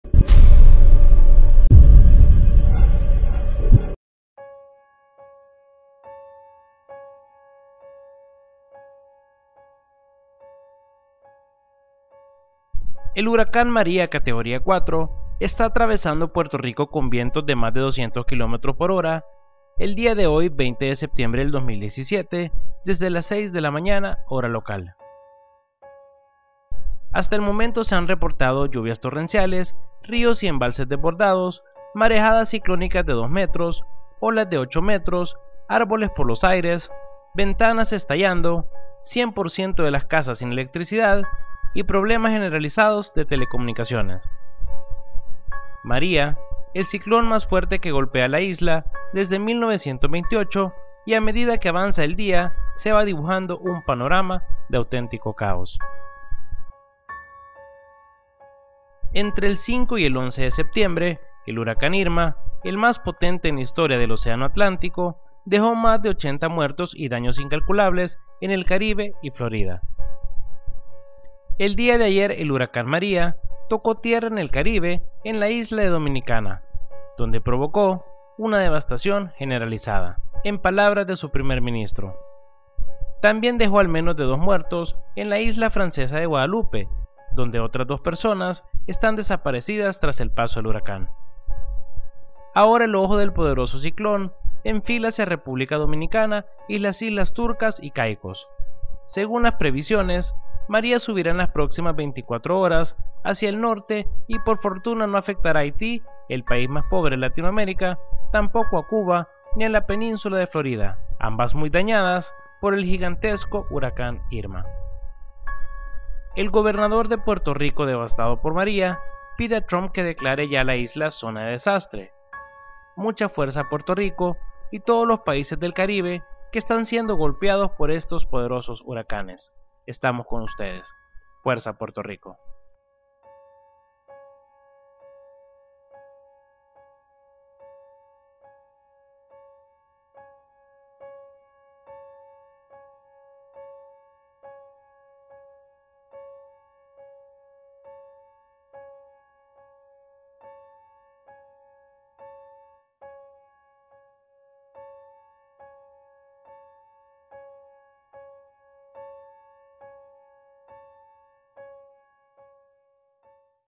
María atraviesa Puerto Rico y se convierte en el más fuerte desde 1928
Hasta el momento se han reportado lluvias torrenciales, ríos y embalses desbordados, marejadas ciclónicas de dos metros, olas de ocho metros, árboles por los aires, 100% de las casas sin electricidad y problemas generalizados de telecomunicaciones.